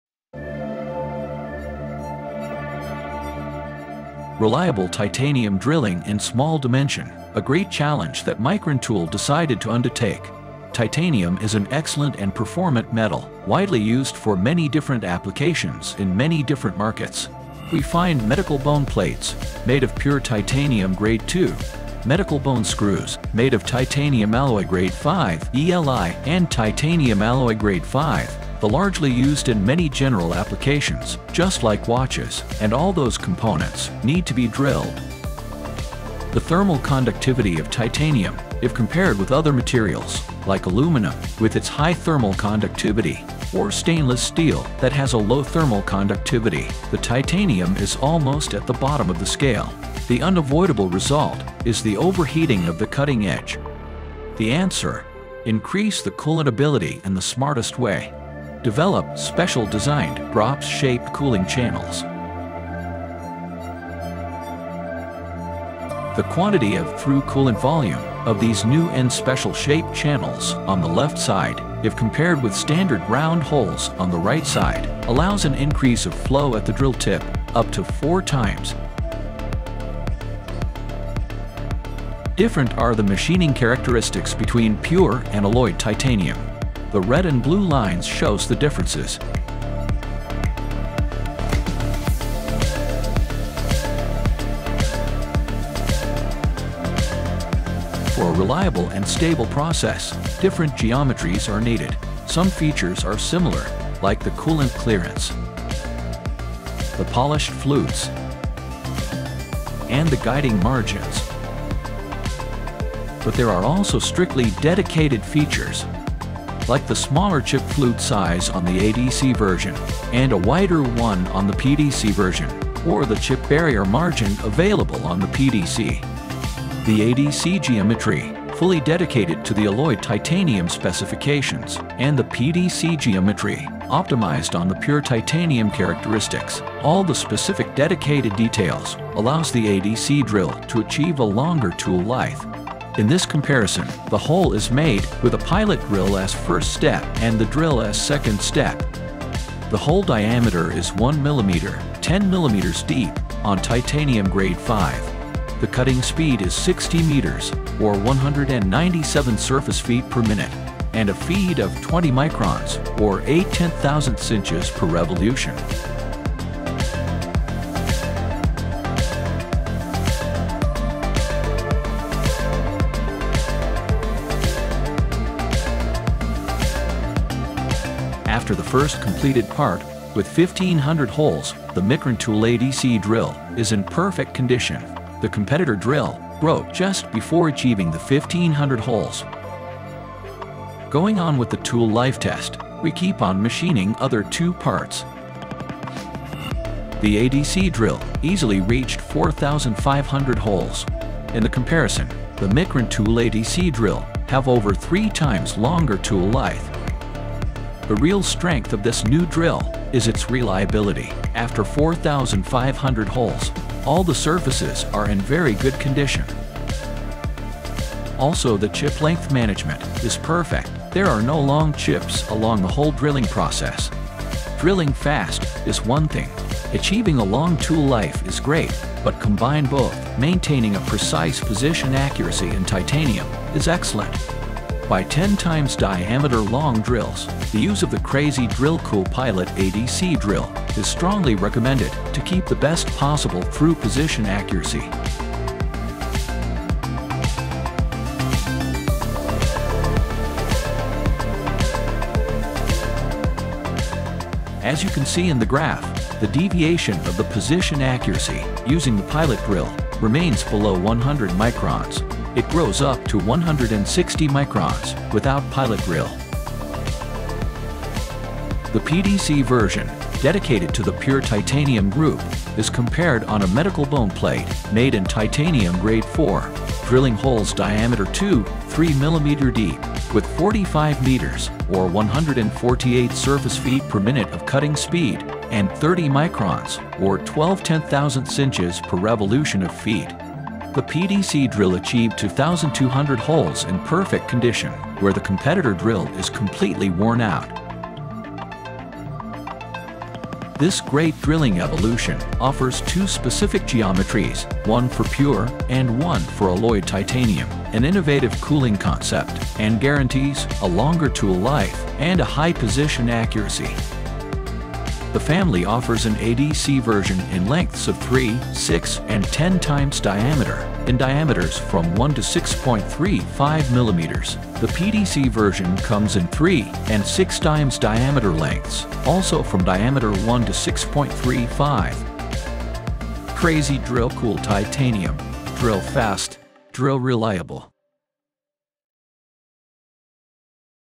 For each Titanium its Drill! - CDC Titanium
Mikron Tool has developed drills perfectly tailored to the respective titanium grades, which can machine titanium safely with high process reliability.

The high-performance material titanium poses a chip removing machining challenge. Also, not all titanium is alike. Depending on whether it is pure titanium or alloyed titanium, the chip removing process behaviour differs.
For these challenges, Mikron Tool has developed drills perfectly tailored to the respective titanium grades, which can machine titanium safely, with higher cutting performance results, longer tool lives and excellent hole quality.
Who wants to machine titanium economically with the highest process reliability, you won’t get far with universal tools. What works for pure titanium reaches its limits with its alloys, and vice versa.
That is why the design of a new high-performance tool requires a detailed analysis of the different titanium grades and the specific characteristics of their composition, which result in significant machining differences.

Mikron Tool has considered these differences in the design of the latest generation of titanium drills and has identified a material-specific cutting-edge geometry as the current best solution for the design of such drills. The result is the new CrazyDrill Cool Titanium ATC/PTC, available with ATC geometry for titanium alloys and PTC geometry for pure titanium.
The new CrazyDrill Cool Titanium series is offered in the diameter range of 1 - 6.35 mm (.039" – 1/4"). For pure titanium, Mikron Tool developed short drills with 3 x d and drills with 6 x d. For titanium alloys, Mikron Tool relies on drills with 6 x d and 10 x d in combination with a pilot drill.

Discover more!